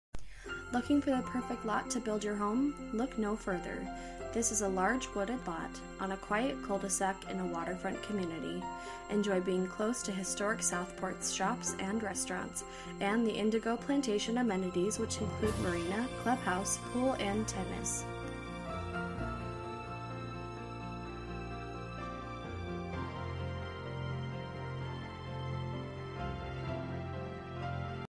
Homes for sale - 673 Carolina Bay Court, Southport, NC 28461
Looking for the perfect lot to build your home? Look no further! This is a large wooded lot on a quiet cul-de-sac in a waterfront community. Enjoy being close to Historic Southport's shops and restaurants, and the Indigo Plantation amenities which include: Marina, clubhouse, pool, and tennis.

Bedrooms: 0
Bathrooms: 0.00
Square Feet: 0
Price: $100,000

For more information about this property, please contact Dennis & Penny Krueger at or . You can also text 5797303 to 67299.